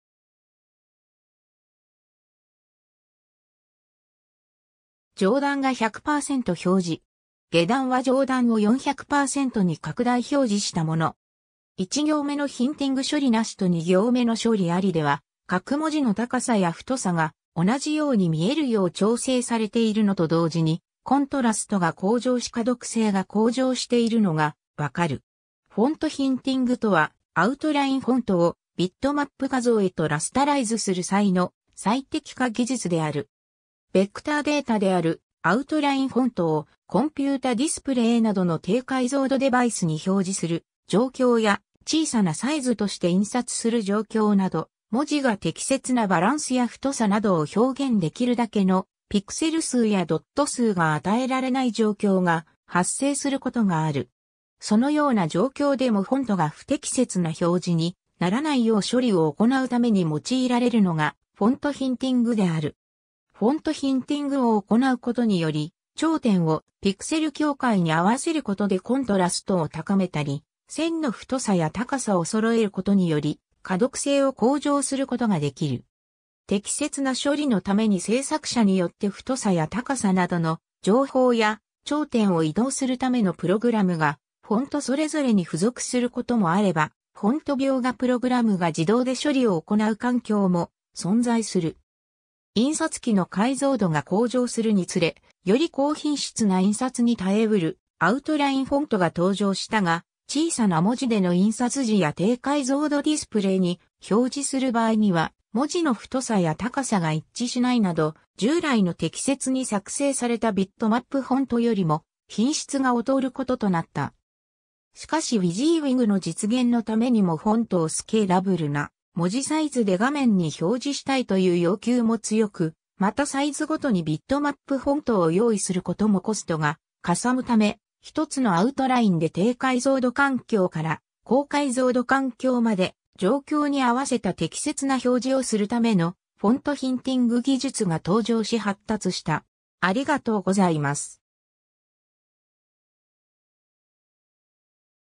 フォントヒンティング
上段が100%表示。
下段は上段を400%に拡大表示したもの。
1行目のヒンティング処理なしと2行目の処理ありでは各文字の高さや太さが同じように見えるよう調整されているのと同時にコントラストが向上し可読性が向上しているのがわかる。
 フォントヒンティング (英: font hinting) とは、アウトラインフォントをビットマップ画像へとラスタライズする際の最適化技術である。
 ベクターデータであるアウトラインフォントをコンピュータディスプレイなどの低解像度デバイスに表示する状況や小さなサイズとして印刷する状況など、文字が適切なバランスや太さなどを表現できるだけのピクセル数やドット数が与えられない状況が発生することがある。
 そのような状況でもフォントが不適切な表示にならないよう処理を行うために用いられるのがフォントヒンティングである。
 フォントヒンティングを行うことにより頂点をピクセル境界に合わせることでコントラストを高めたり、線の太さや高さを揃えることにより可読性を向上することができる。
適切な処理のために製作者によって太さや高さなどの情報や、頂点を移動するためのプログラムがフォントそれぞれに付属することもあれば、フォント描画プログラムが自動で処理を行う環境も存在する。
 印刷機の解像度が向上するにつれ、より高品質な印刷に耐えうるアウトラインフォントが登場したが、小さな文字での印刷時や低解像度ディスプレイに表示する場合には文字の太さや高さが一致しないなど、従来の適切に作成されたビットマップフォントよりも品質が劣ることとなった。
 しかしWYSIWYGの実現のためにもフォントをスケーラブルな文字サイズで画面に表示したいという要求も強く、またサイズごとにビットマップフォントを用意することもコストがかさむため、1つのアウトラインで低解像度環境から高解像度環境まで、状況に合わせた適切な表示をするためのフォントヒンティング技術が登場し発達した。